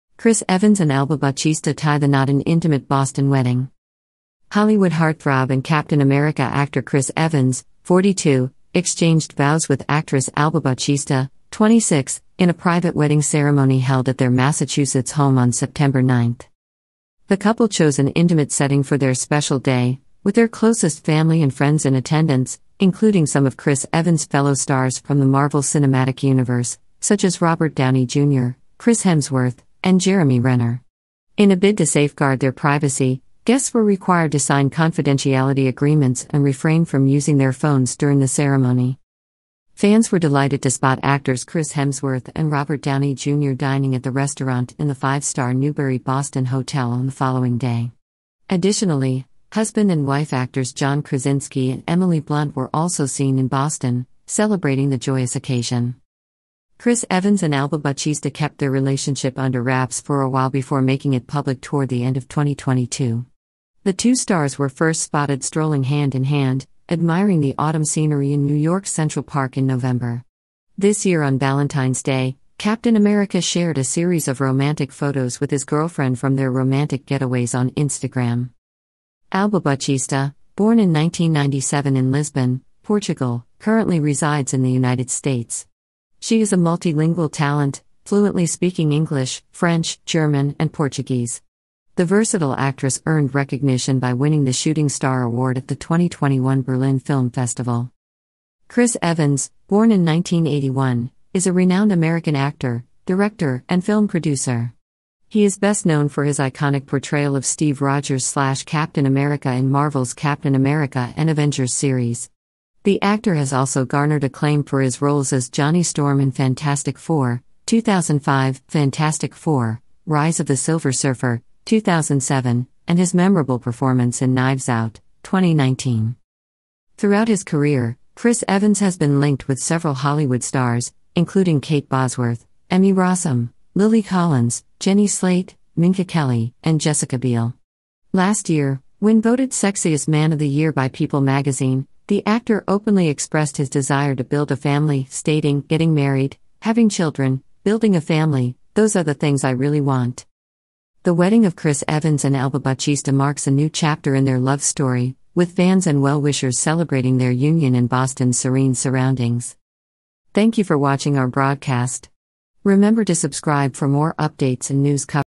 Chris Evans and Actress's Whirlwind Romance Culminates in Wedding Bliss
In a small ceremony in Boston, Chris Evans and Alba Baptista exchange vows.

Chris Evans, 42, the star of the Marvel Cinematic Universe's "Captain America," married Alba Baptista, 26, on September 9 in a small, intimate ceremony at their home in Massachusetts.

The wedding was small and personal, with just immediate family and close friends present in addition to Chris Evans' MCU co-stars Robert Downey Jr., Chris Hemsworth, and Jeremy Renner. To protect the couple's privacy, they had attendees sign nondisclosure agreements and ask that they turn off their phones during the service.

The next day, fans flocked to the restaurant at the five-star Newbury Boston hotel where they spotted celebrities Chris Hemsworth and Robert Downey Jr. John Krasinski and Emily Blunt, who are married, were also spotted in Boston for the celebration.

It wasn't until the latter half of 2022 that the world learned of Chris Evans and Alba Baptista's romance. The earliest known sighting of the two stars together was in Central Park, New York, in the month of November. On Instagram, "Captain America" shared a series of intimate images from Valentine's Day this year with his partner.

Originally from Lisbon, Portugal, Alba Baptista was born in 1997 and now makes her home in the United States. She is a talented polyglot who is conversant in four languages. The multitalented actress was honored with the Shooting Star prize at the 2021 Berlin Film Festival.

American actor, director, and producer Chris Evans was born in 1981. His role of Steve Rogers/Captain America in the Captain America and Avengers film series made him a household name. The actor is well-known for his memorable performance in "Knives Out" (2019) and his supporting appearances as Johnny Storm in "Fantastic Four" (2005) and "Fantastic Four: Rise of the Silver Surfer" (2007).

Chris Evans's career has been littered with rumors of romantic involvement with a number of A-list actresses, including Kate Bosworth, Emmy Rossum, Lily Collins, Jenny Slate, Minka Kelly, and Jessica Biel. When asked about his desire to start a family last year, the actor who had just been named People magazine's "Sexiest Man of the Year" said, "Getting married, having children, building a family, those are the things I really want."

Fans and well-wishers celebrated the beginning of a new chapter in the love story of Chris Evans and Alba Baptista with their wedding in the tranquil setting of Boston.

We appreciate you tuning in to our show.  Don't forget to sign up to receive further notifications and news coverage.